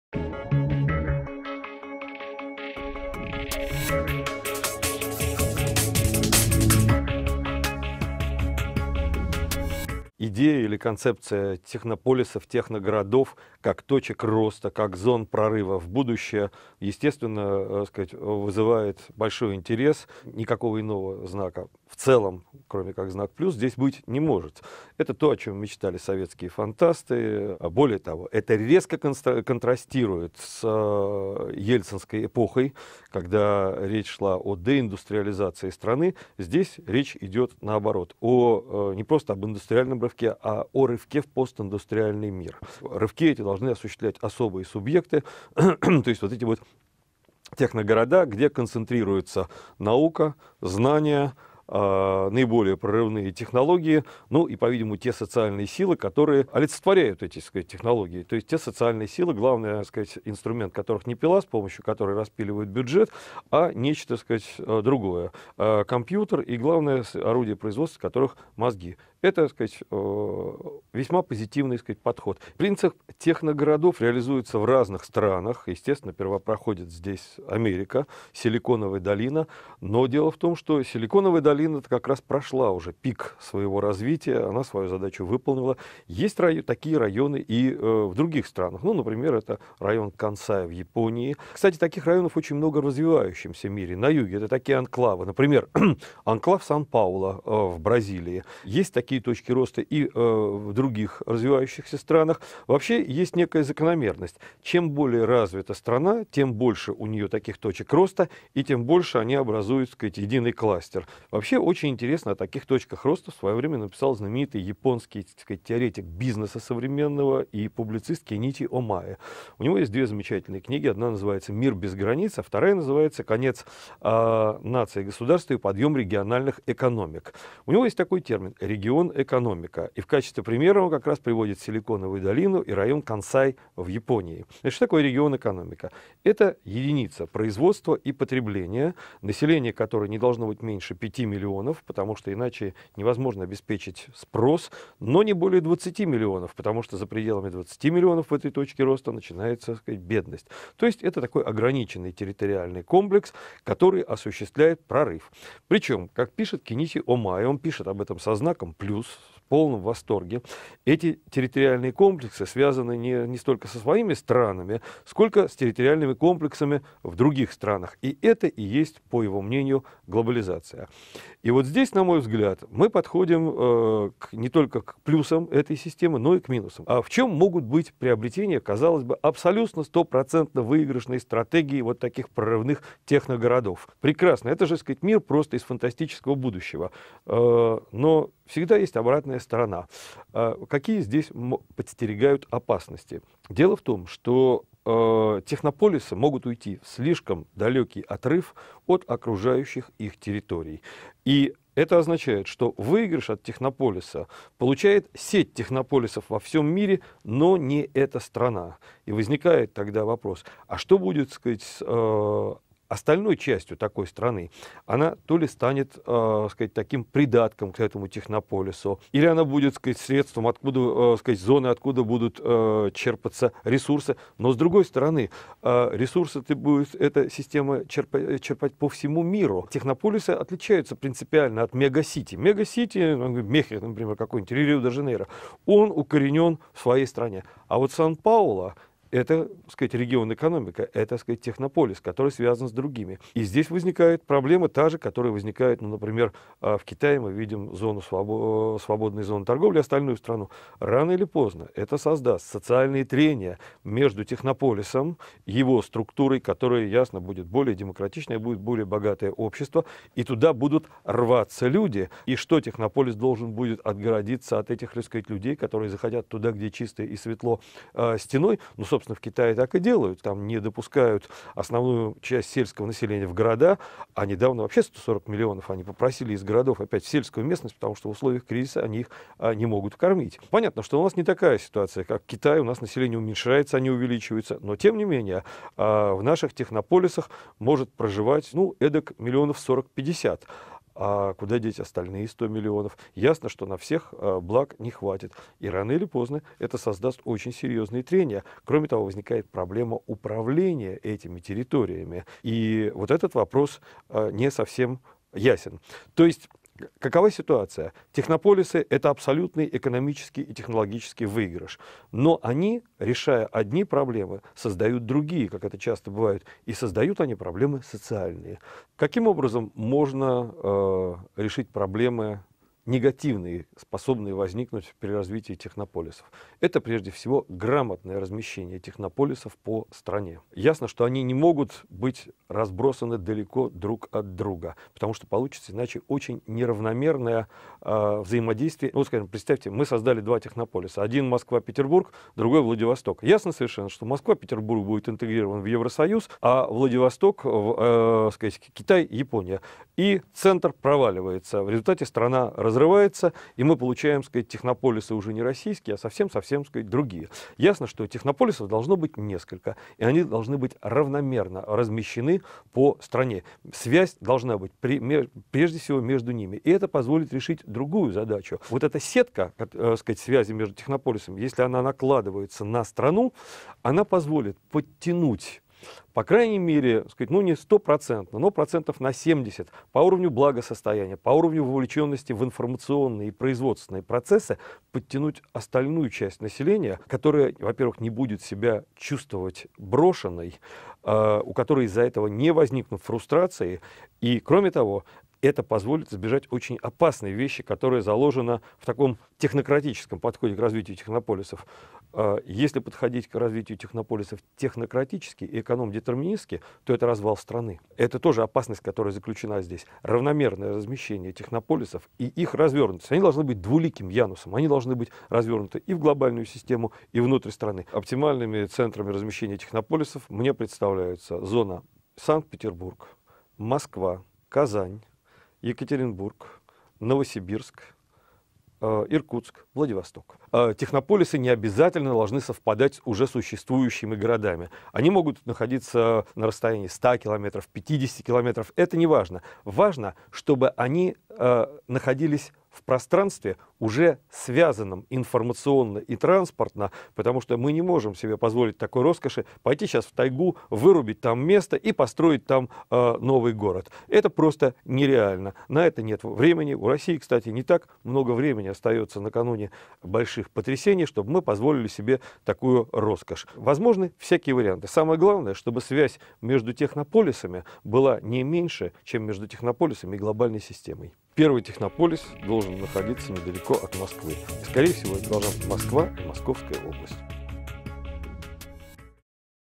Фурсов - Какие города станут Технополисами? - Russia.ru
Проекты техногородов зачастую не учитывают, что с их созданием сопряжено немало опасностей. Может возникнуть неравновесие между особыми городами и обычными, поддержание инфраструктуры перетянет лучшие кадры из окружающих поселений. Поэтому Андрей Фурсов говорит о том, что нужно учитывать все факторы, но и результат может быть отличным.

© ООО "Популярная литература"